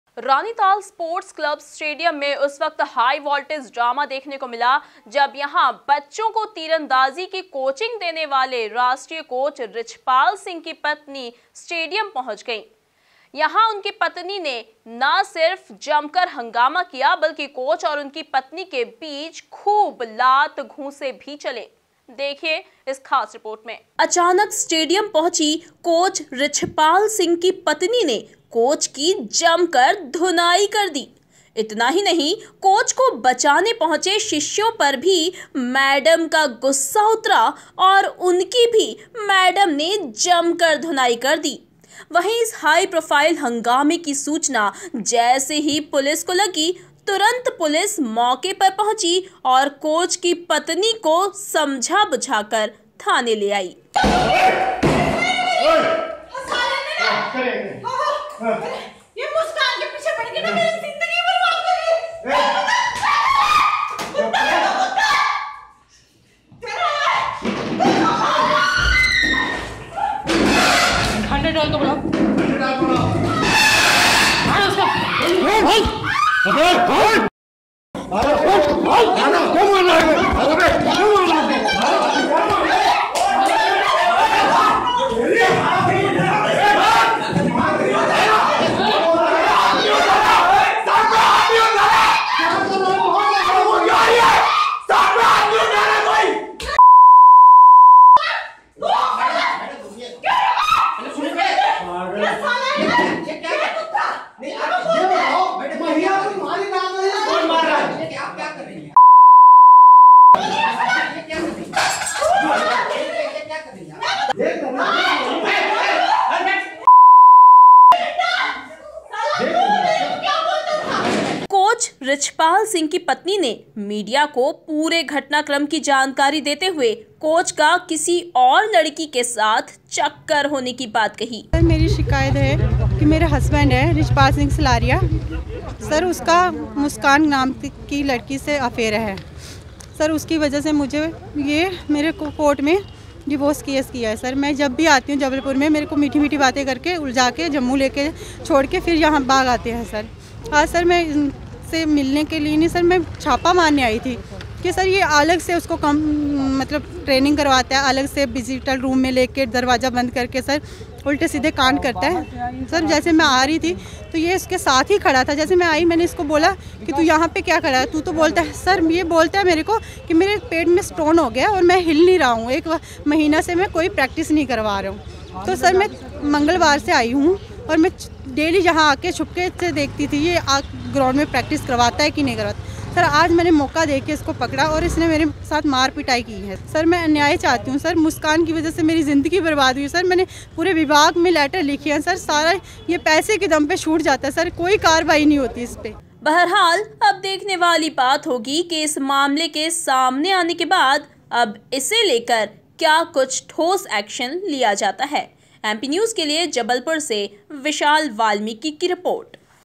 जबलपुर के स्पोर्ट्स स्टेडियम में हाईवोल्टेज ड्रामा, कोच की पत्नी ने जमकर किया हंगामा! MP News
जबलपुर के स्पोर्ट्स स्टेडियम में हाईवोल्टेज ड्रामा, कोच की पत्नी ने जमकर किया हंगामा! MP News Jabalpur